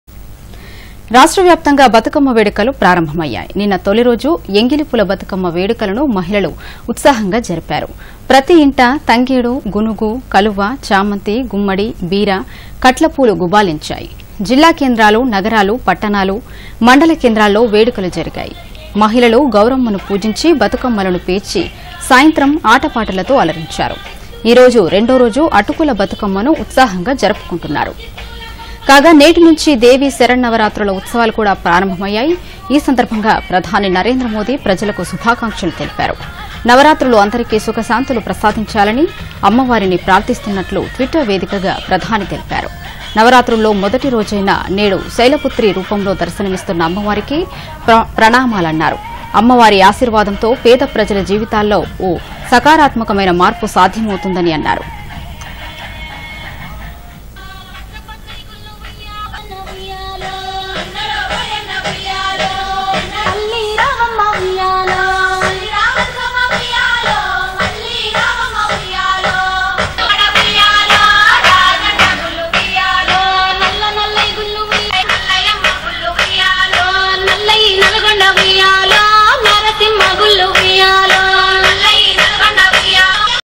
రాష్ట్ర వ్యాప్తంగా బతుకమ్మ వేడుకలు ప్రారంభమయ్యాయి _ నరేంద్ర మోదీ ప్రజలకు శుభాకాంక్షలు తెలిపారు.
రాష్ట్ర వ్యాప్తంగా బతుకమ్మ వేడుకలు ప్రారంభమయ్యాయి. నిన్న తొలిరోజు ఎంగిలిపూల బతుకమ్మ వేడుకలను మహిళలు ఉత్సాహంగా జరిపారు. ప్రతీ ఇంటా తంగేడు, గునుగు, కలువ, చామంతి, గుమ్మడి, బీర, కట్ల పూలు గుబాళించాయి. జిల్లా కేంద్రాలు, నగరాలు, పట్టణాలు, మండల కేంద్రాల్లో వేడుకలు జరిగాయి. మహిళలు గౌరమ్మను పూజించి బతుకమ్మలను పేర్చి సాయంత్రం ఆటపాటలతో అలరించారు. ఈ రోజు రెండో రోజు అటుకుల బతుకమ్మను ఉత్సాహంగా జరుపుకుంటున్నారు.

కాగా.. నేటి నుంచి దేవీ శరన్నవరాత్రుల ఉత్సవాలు కూడా ప్రారంభమయ్యాయి. ఈ సందర్భంగా  ప్రధాని నరేంద్ర మోదీ ప్రజలకు శుభాకాంక్షలు తెలిపారు. నవరాత్రులు అందరికి సుఖ:శాంతులు ప్రసాదించాలని అమ్మవారిని ప్రార్ధిస్తున్నట్లు ట్విట్టర్ వేదికగా ప్రధాని తెలిపారు. నవరాత్రుల్లో మొదటి రోజైన నేడు శైలపుత్రి రూపంలో దర్శనమిస్తున్న అమ్మవారికి ప్రణామాలన్నారు. . అమ్మవారి ఆశీర్వాదంతో పేద ప్రజల జీవితాల్లో ఓ సకారాత్మకమైన మార్పు సాధ్యమవుతుందని అన్నారు.
..పాజ్‌...